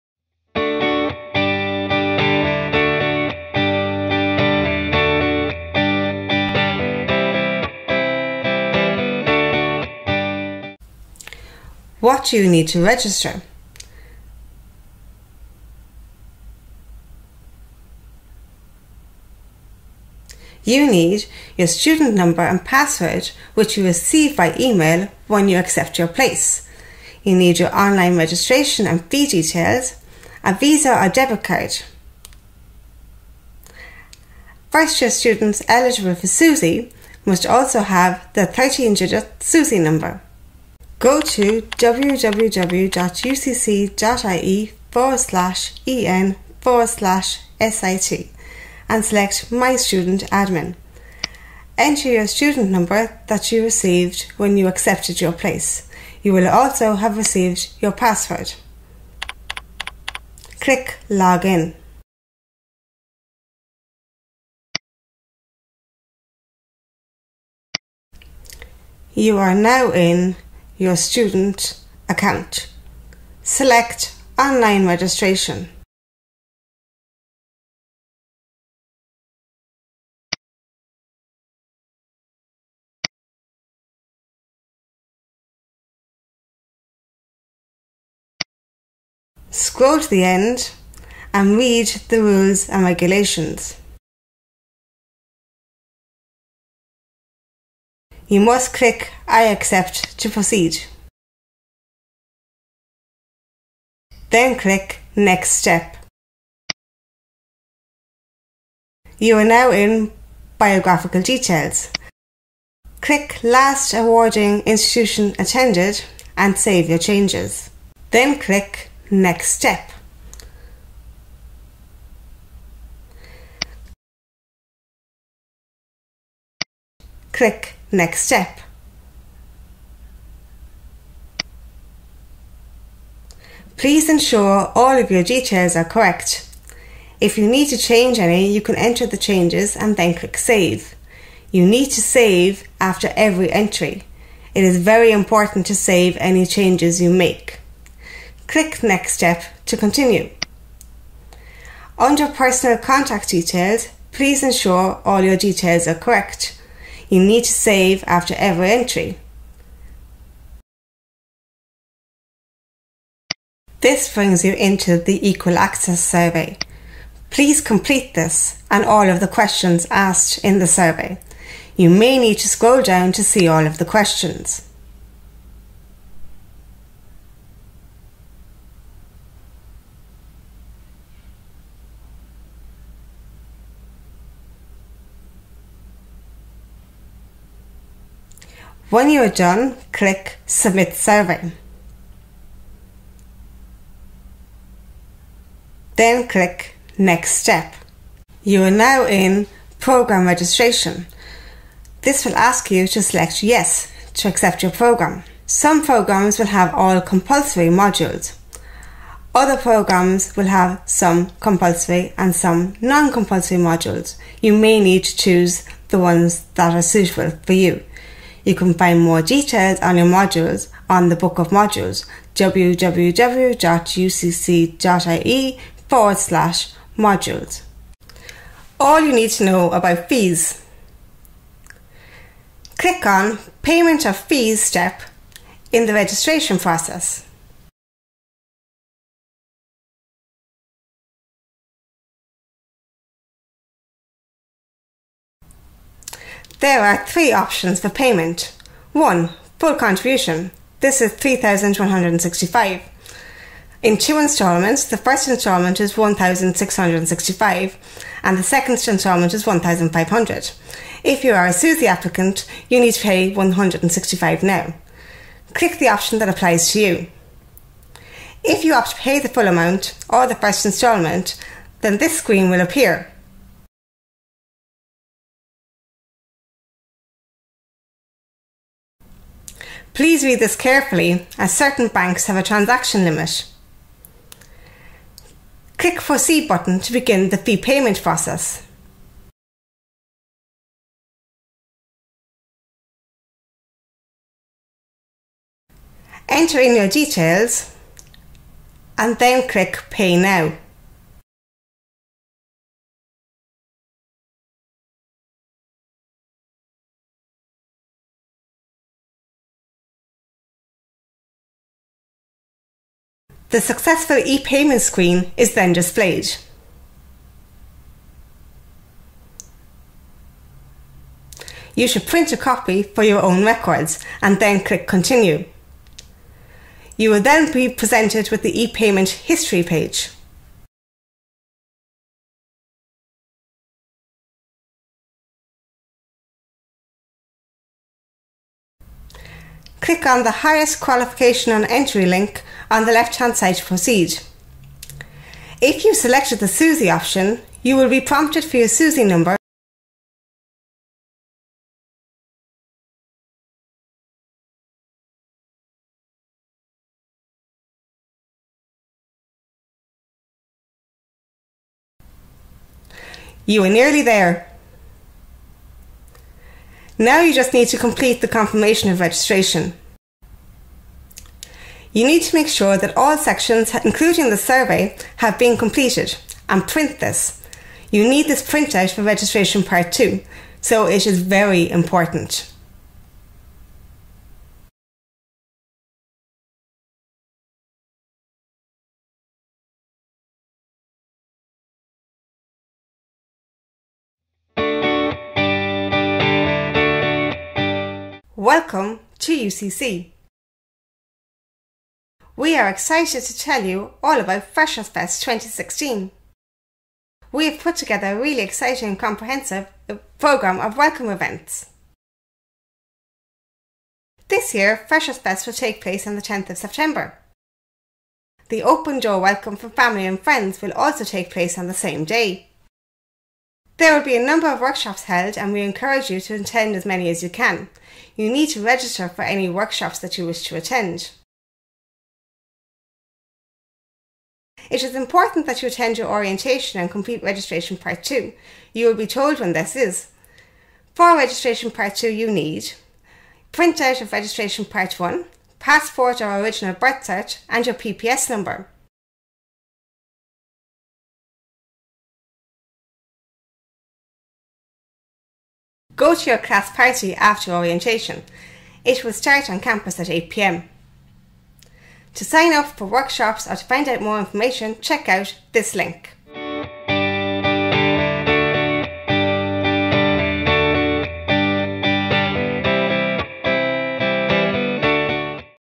Registering as an Undergraduate UCC Student
Darrelle Keegan, DSS, University College Cork guides you registering as an undergraduate student in UCC.